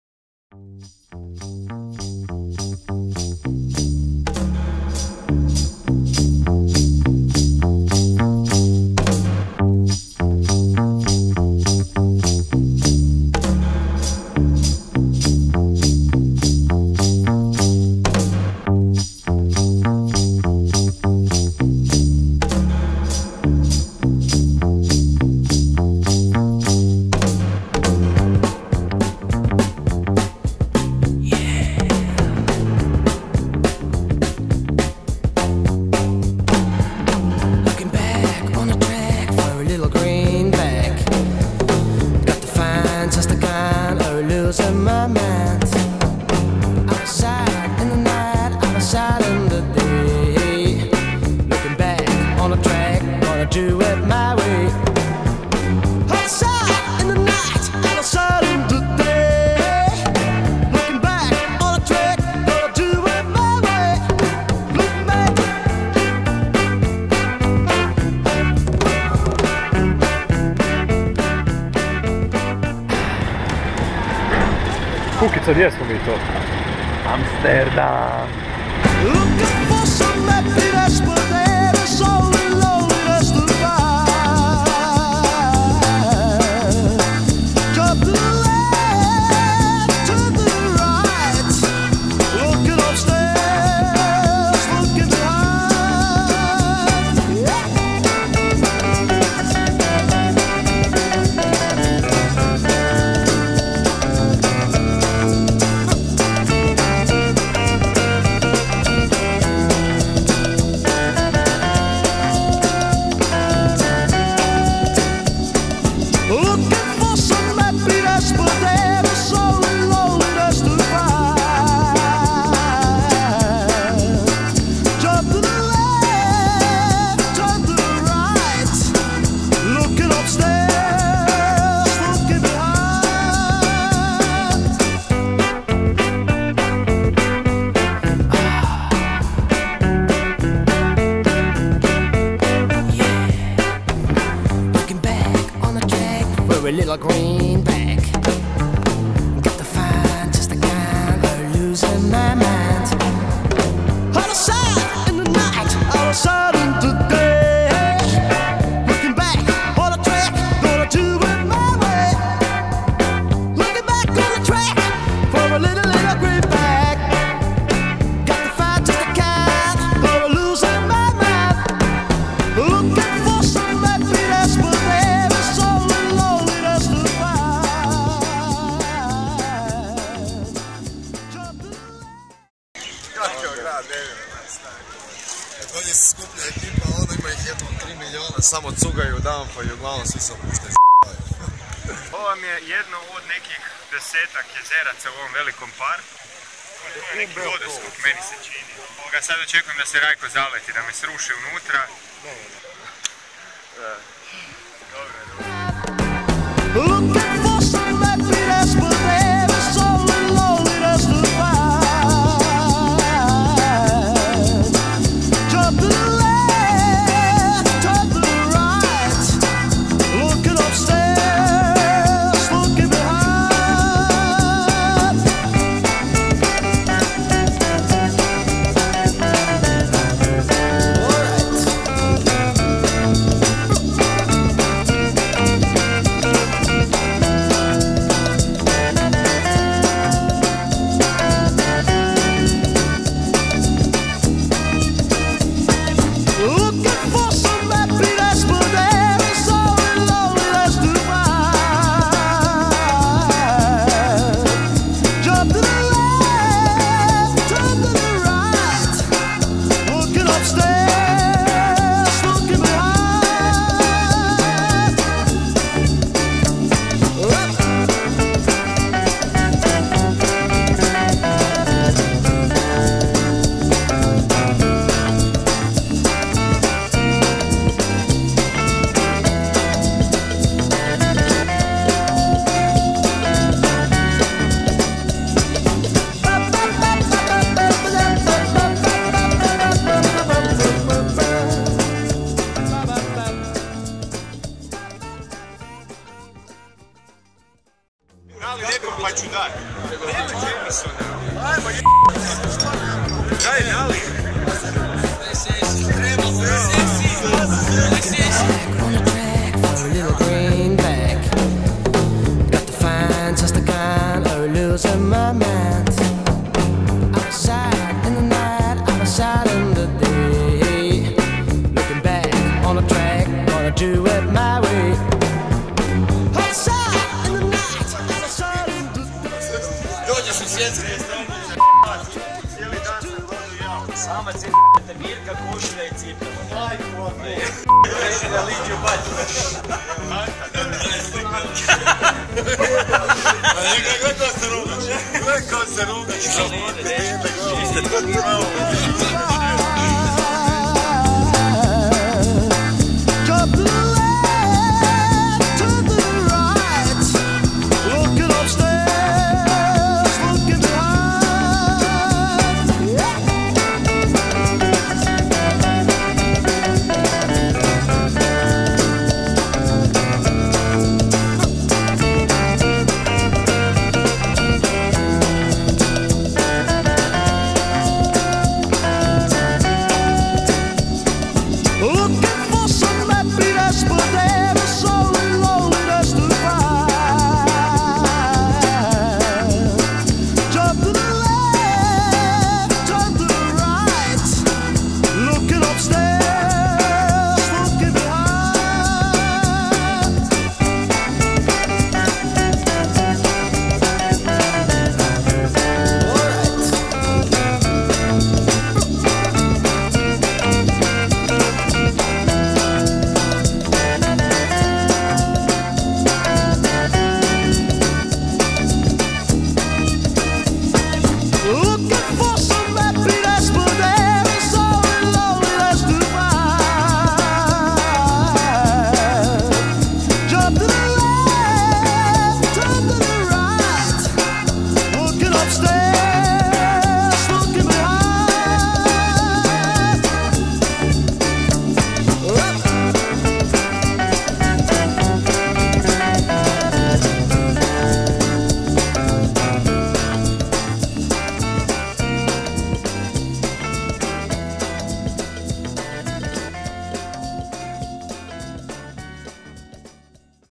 Trip to Amsterdam
“Some tourists think Amsterdam is a city of sin, but in truth it is a city of freedom. And in freedom, most people find sin.”

by John Green

One team, 12 people, 5 days in Amsterdam.

Recorded with GoPro Hero 3 and Sony Xperia Z3 Compact.